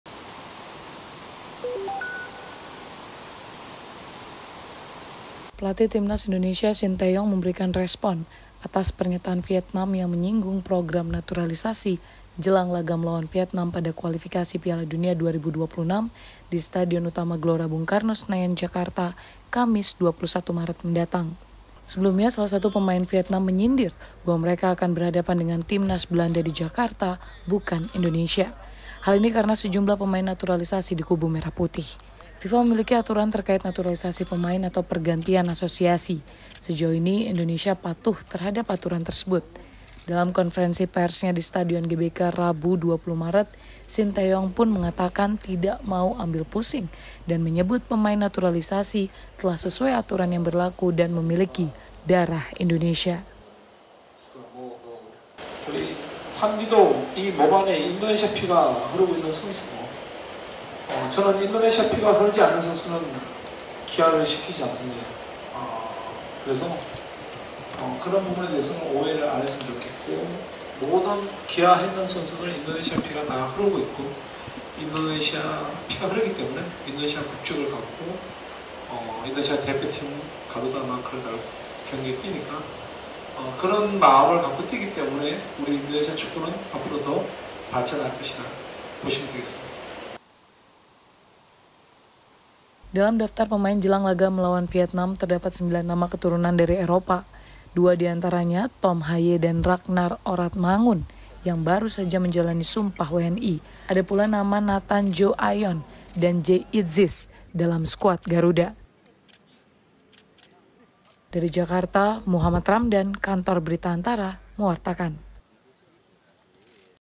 Shin Tae-yong: Pemain yang dinaturalisasi punya darah Indonesia
ANTARA - Pelatih timnas Indonesia Shin Tae-yong memberikan respon terkait naturalisasi jelang laga melawan Vietnam di Stadion Utama GBK, Senayan, Jakarta, Kamis (23/3). Dalam konferensi pers satu hari jelang laga, Shin Tae-young menegaskan pemain naturalisasi memiliki darah Indonesia dan layak mengenakan lambang garuda di dada. (Muhammad Ramdan/Rizky Bagus Dhermawan/I Gusti Agung Ayu N)